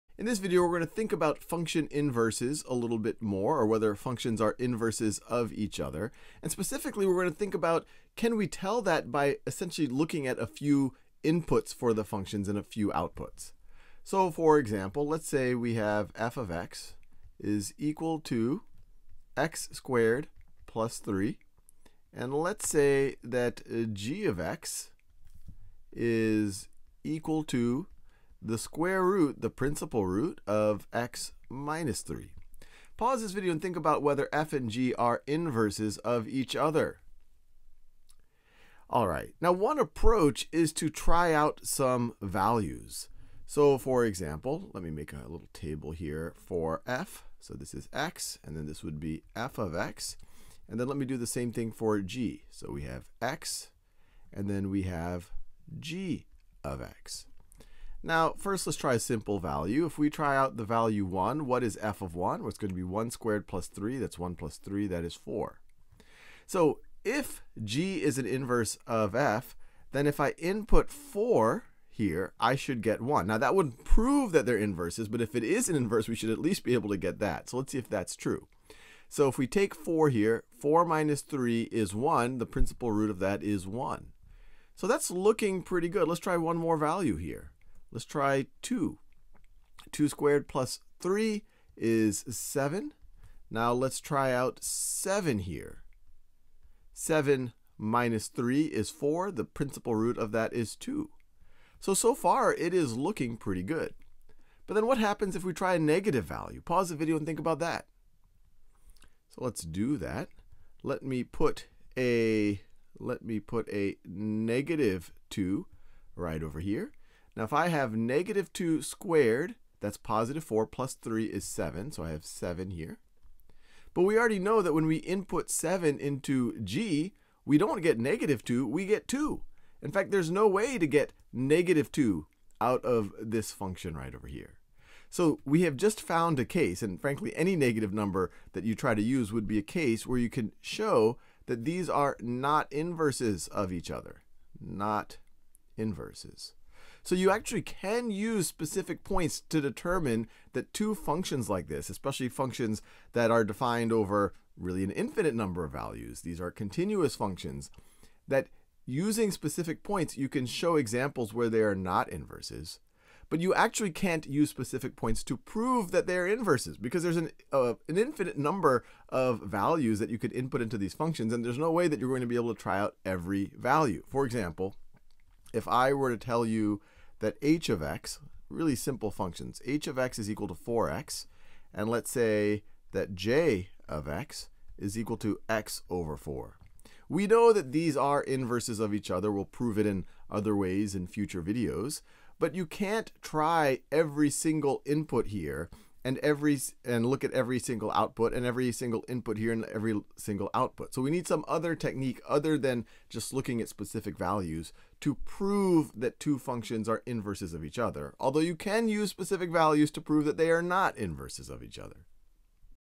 Using specific values to test for inverses | Precalculus | Khan Academy
Are two functions inverses? Even one counterexample can show that they are not. How many examples would it take to show that they really are inverses?